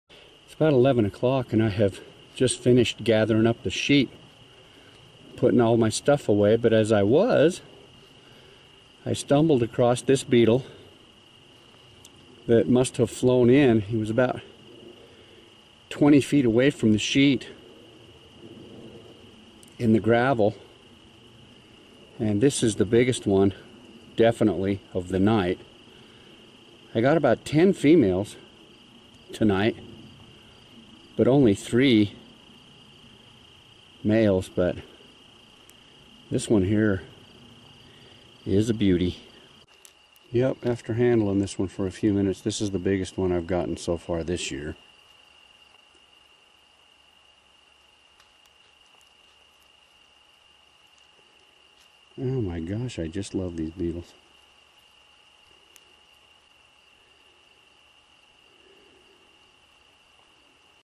Dynastes Beetle
Male Dynastes granti beetle. Collected in southern Utah. Showing size and color as well as horns.